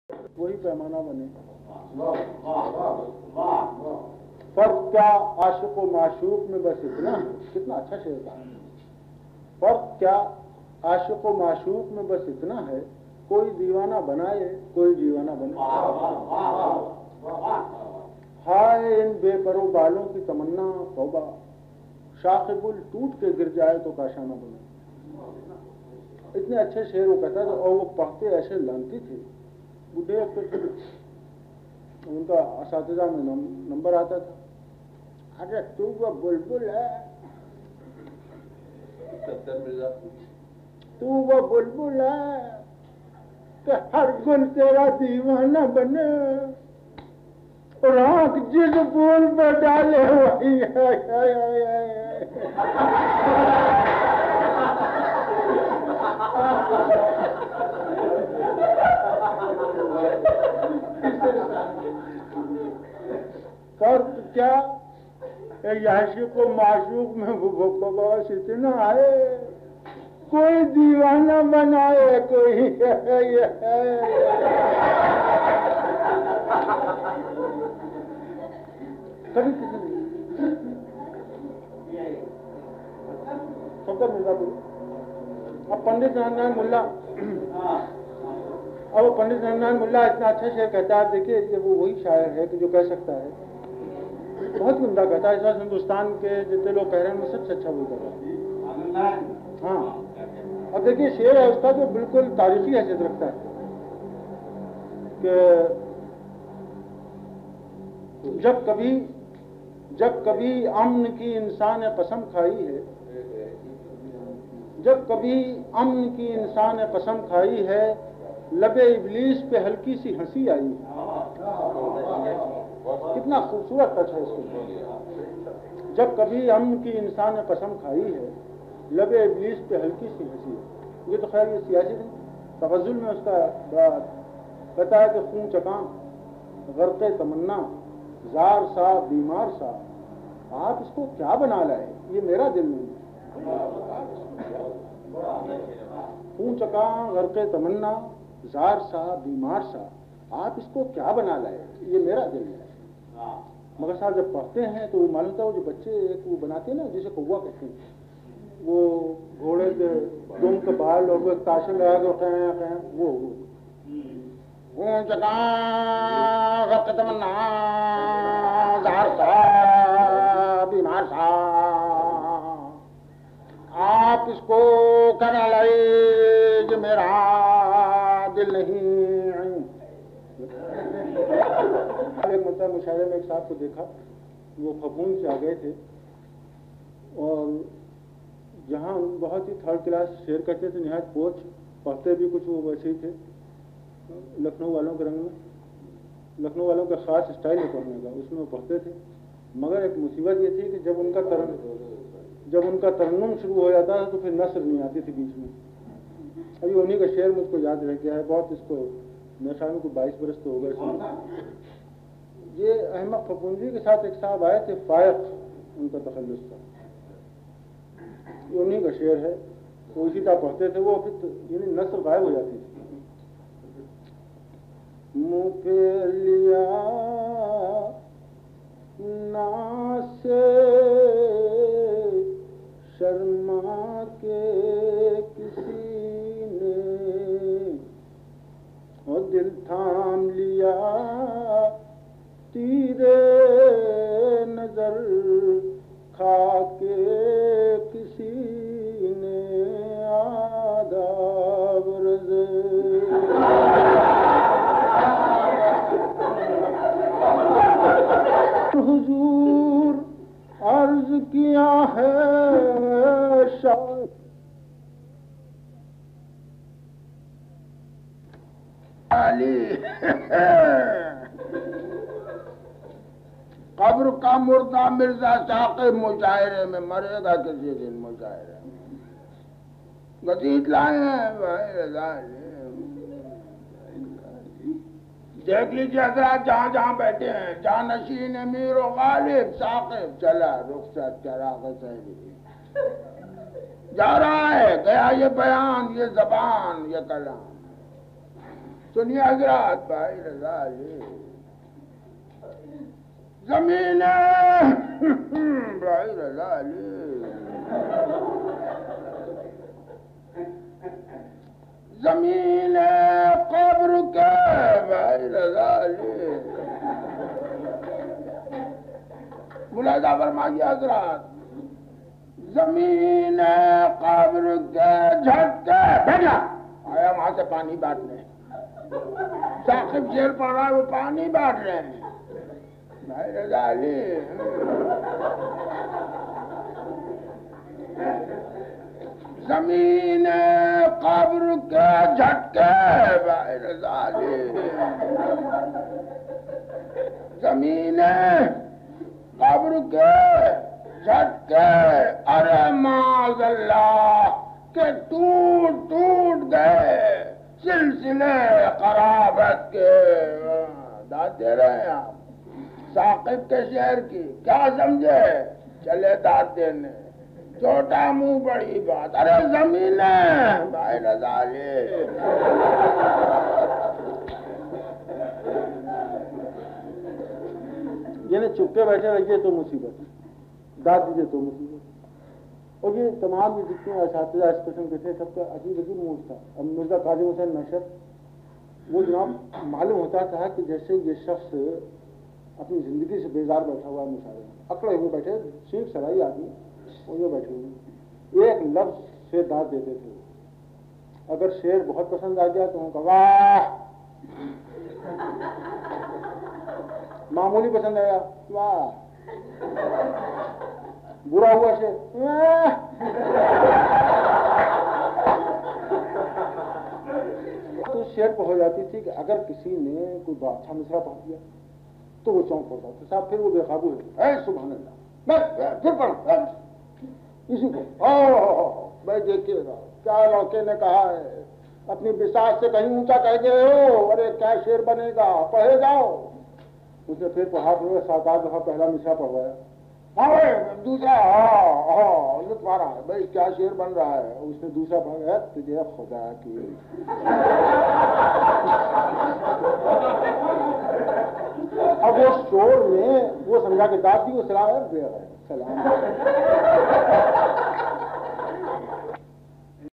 Shaukat Thanvi - Poets
Shaukat Thanvi (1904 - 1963)  (real name Muhammad Umar) was a renowned satirist, writer, poet, journalist, and broadcaster, first in India and then in Pakistan. Among his many talents was that of a voice actor and mimic. In this recording from a pribate event c. 1960, he mimics several Urdu poets, including Anand Narain Mulla and Saqib Lakhnavi.